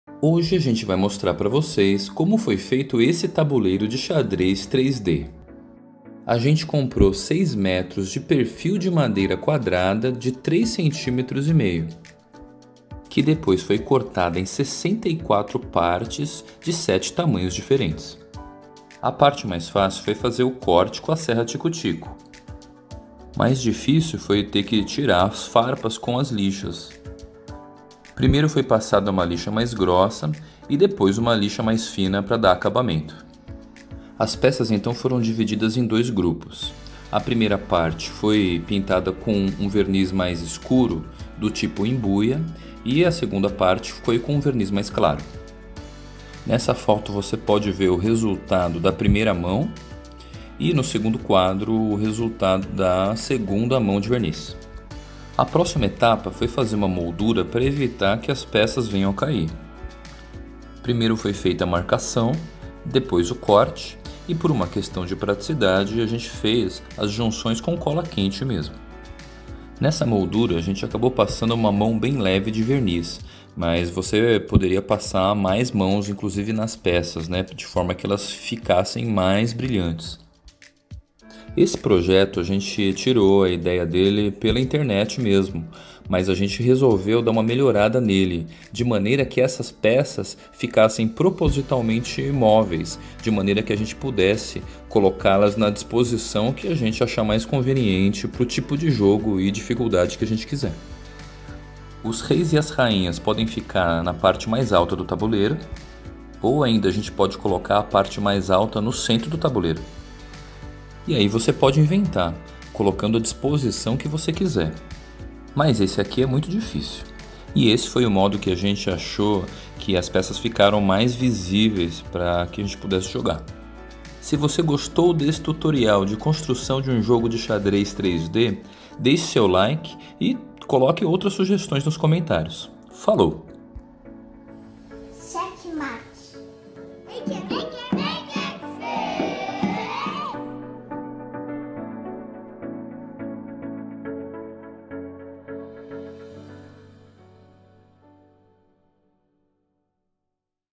Construção de um tabuleiro 3D de xadrez
Conteúdo: tutorial de construção de um tabuleiro 3D de xadrez.

Materiais utilizados:
- 6 m de perfil de madeira quadrada de 3,5 cm;
- 1,5 m de madeira para a moldura de 1 x 2 cm;
- 1 base de madeira de 40 x 60 cm;
- 1 latinha de verniz claro;
- 1 latinha de verniz imbuia;
- 2 rolinhos de pintura;
- 5 lixas diversas;
- 1 serra tico-tico;
- 1 pistola de cola quente.

Do it yourself (DIY), ou seja, faça você mesmo!

Apresentadores: Marcio Koba e Evelyn